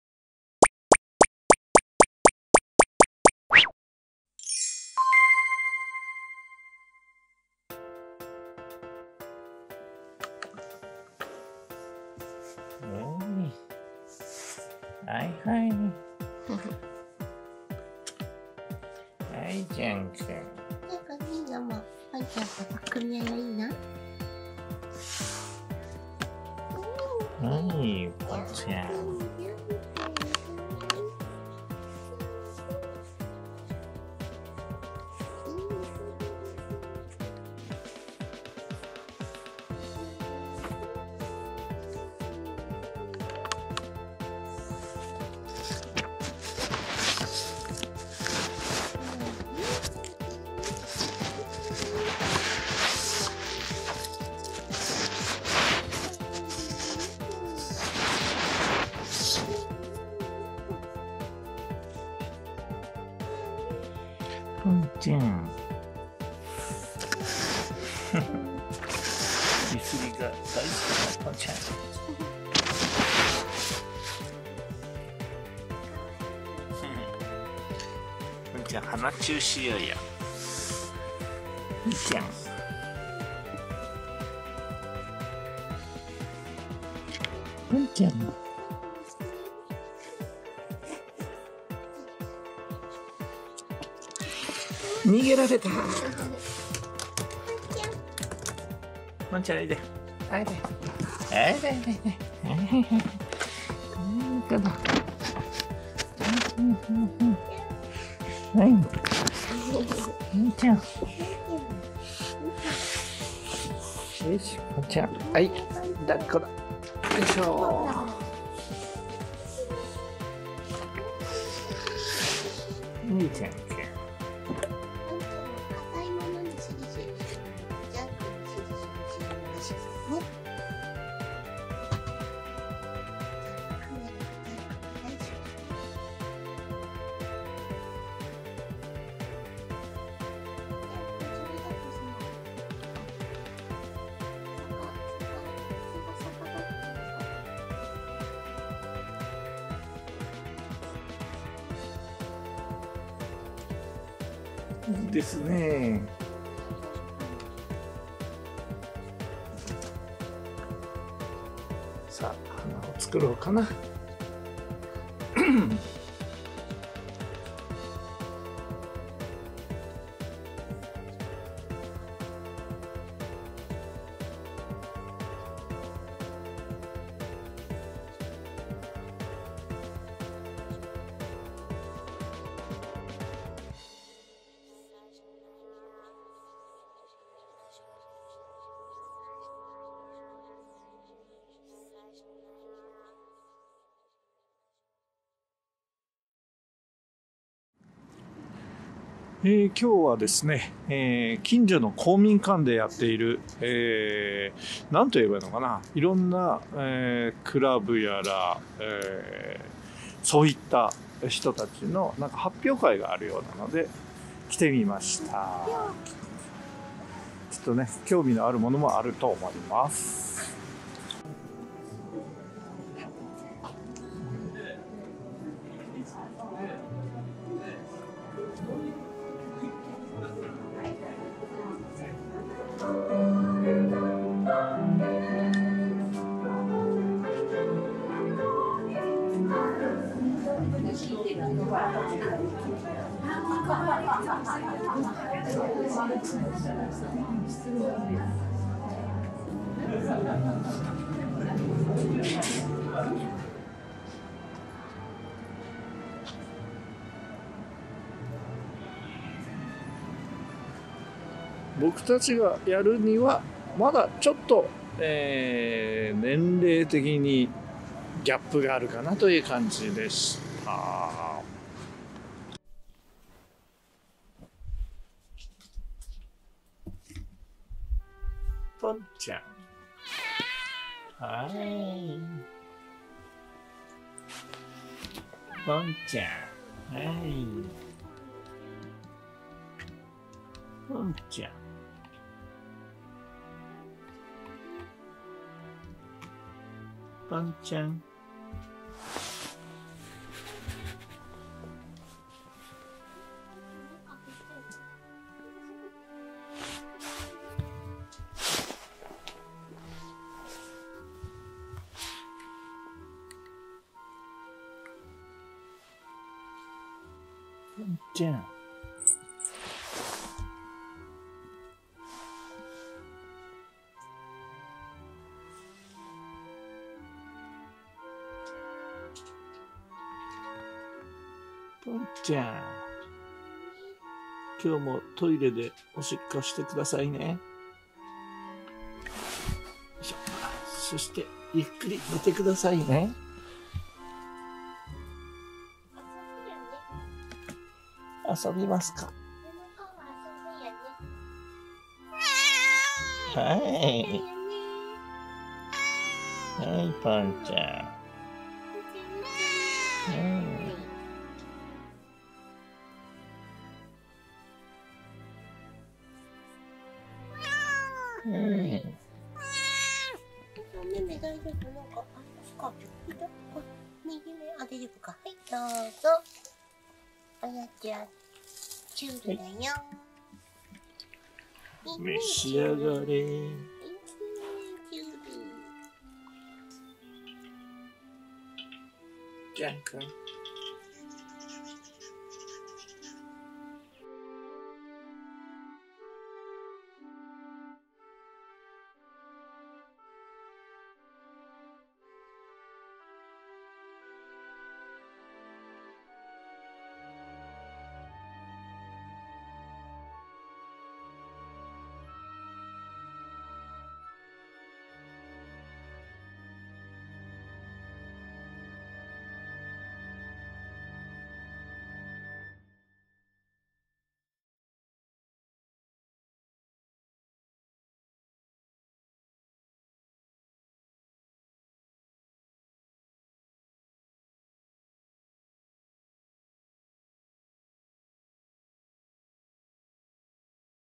【Jean & Pont 1636】この街の文化を見に公民館の文化祭に行って来ました 2019/2/10
街の公民館で様々な文化的な活動の発表会、文化祭のようなものをやっているというので高齢の母も一緒に出かけて見ました。
絵画やフラワーデザイン、合唱や詩吟、俳句、書道など様々な作品がちょっとだけ見られるようになったいましたが、母にはちょうど合いそうな感じです。
ただ僕たち夫婦にはまだちょっとこの欲のない作品には年齢的に到達できておらず、もう少しドロッと欲にまみれた世界の方が良さそうです。